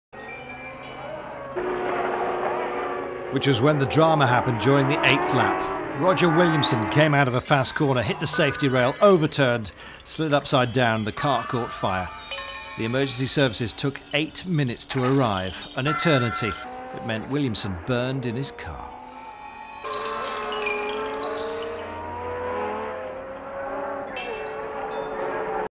Roger Williamson | Fatal Accident | Good Quality
Fatal Accident of Roger Williamson at the Dutch GP 1973